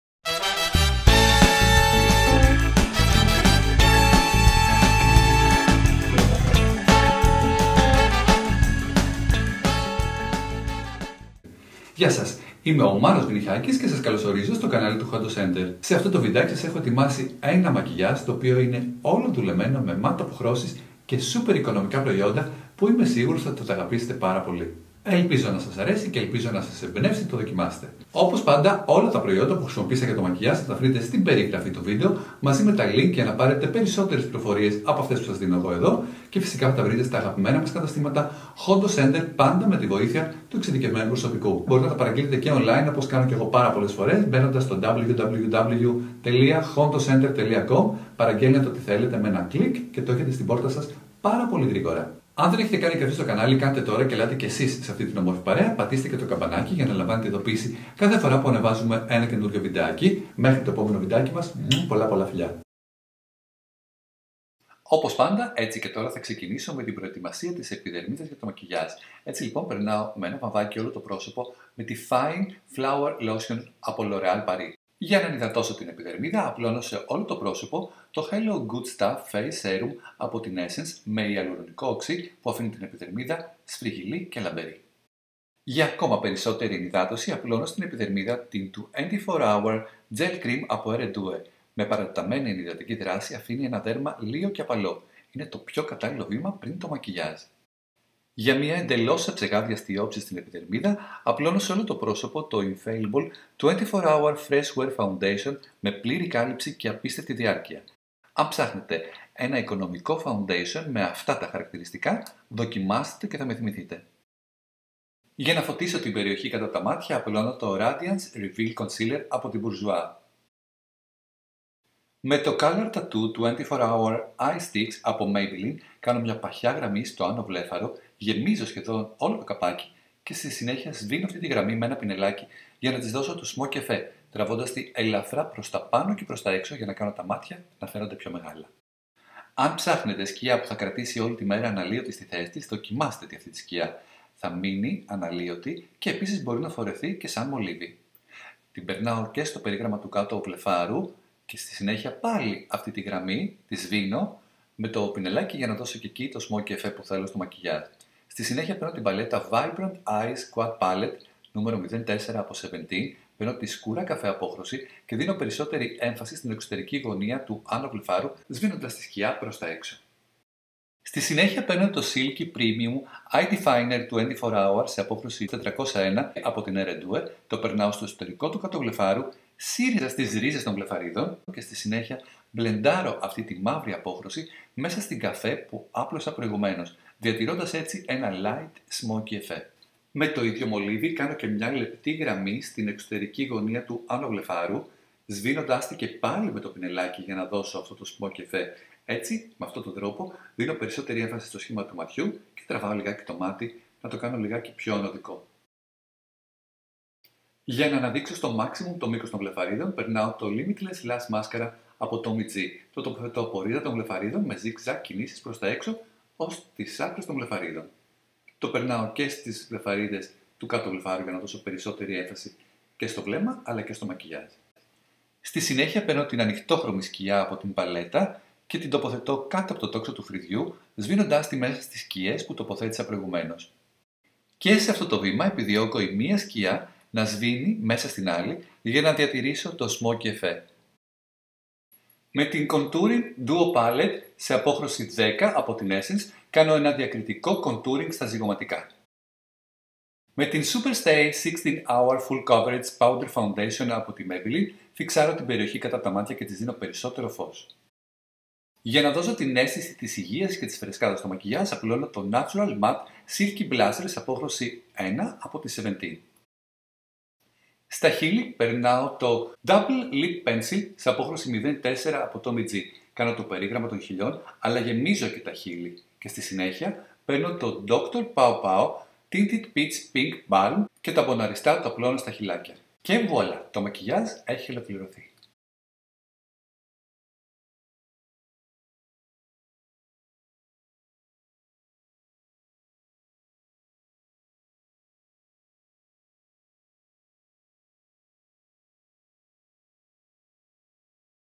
VIVA MATTE MAKE UP by MANOS VYNICHAKIS | HONDOS CENTER
Ο αγαπημένος σας make up expert Μάνος Βυνιχάκης παρουσιάζει ένα μακιγιάζ, το οποίο είναι όλο δουλεμένο με matte αποχρώσεις και super οικονομικά προϊόντα. Το Viva Matte Make up έχει επιμεληθεί όπως πάντα με τα αγαπημένα του προϊόντα μακιγιάζ από τα καταστήματα Hondos Center!

Mano's Viva Matte Make Up ❤

Τα προϊόντα που χρησιμοποιήθηκαν είναι τα ακόλουθα με τη σειρά που παρουσιάζονται:

L'ORÉAL PARIS, Fine Flowers Τονωτική Λοσιόν Για Ξηρή & Ευαίσθητη Επιδερμίδα

ESSENCE, Hello Good Stuff! Serum

L’ORÉAL PARIS, Infaillible 24H Υγρό Μεικάπ για Πλήρη Κάλυψη / 100

BOURJOIS, Radiance Reveal Concealer

MAYBELLINE, Color Tattoo 24h Eye Stix /  25 - I Am Determined

TOMMY G, Limitless Lash Mascara

MAYBELLINE, Super Stay 16H Αδιάβροχη Πούδρα

TOMMY G, Double Lip Pencil / 04

DR. PAWPAW, Tinted Peach Pink Balm